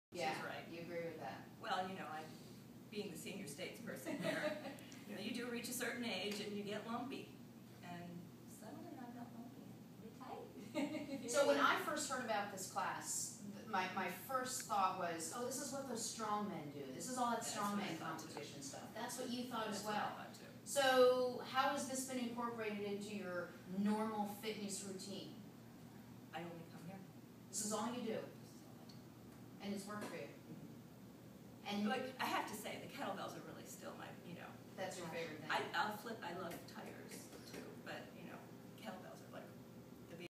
Don't call me Lumpy
Carozza Fitness gang giving their first impression of Strongman